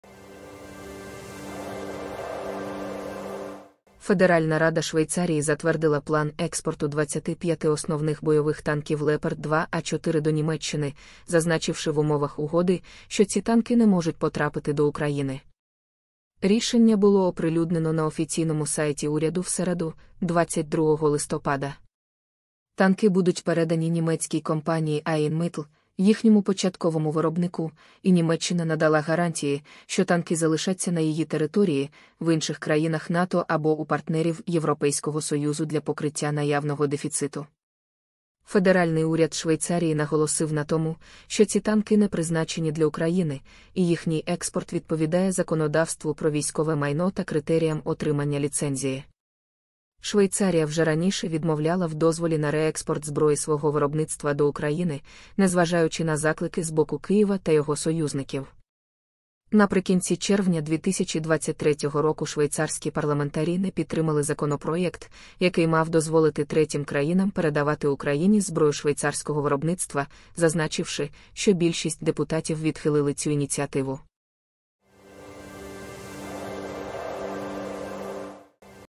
Швейцарія погодила продаж 25 танків Leopard 2A4 Німеччині, із забороною, передачі їх Україні
Джерело інформації  ( Source ):

Збройних сил Швейцарії. Фото з відкритих джерел